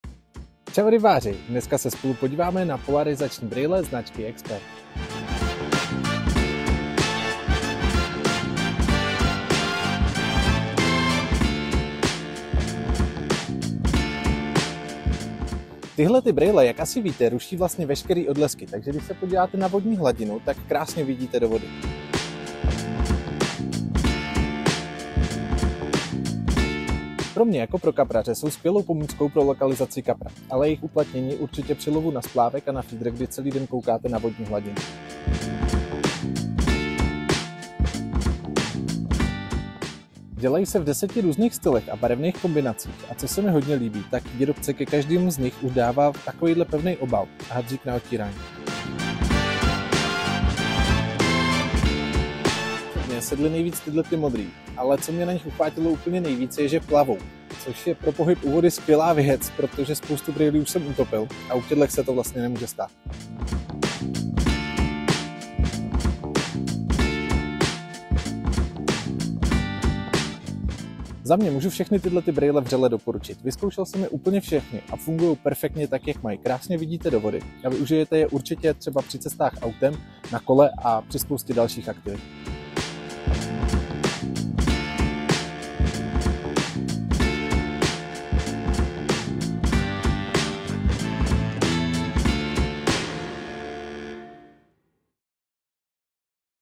Polarizační brýle Expert
Brýle vhodné pro lov kaprů, lov na splávek, lov na feeder a samozřejmě i lov dravců.. Jejich uplatnění naleznete, ale i v běžném životě.